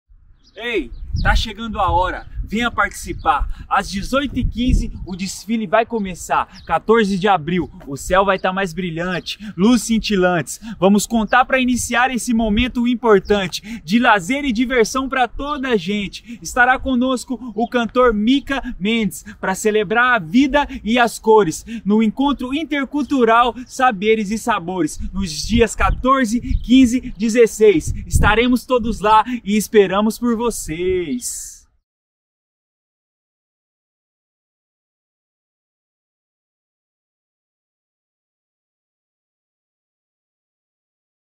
14.º encontro Intercultural Saberes e Sabores
Hoje, a partir das 18 horas, é a abertura oficial do 14.º encontro Intercultural Saberes e Sabores. Venha assistir ao desfile dos países representados e assista ao espetáculo do cantor de kizomba/zouk, Mika Mendes.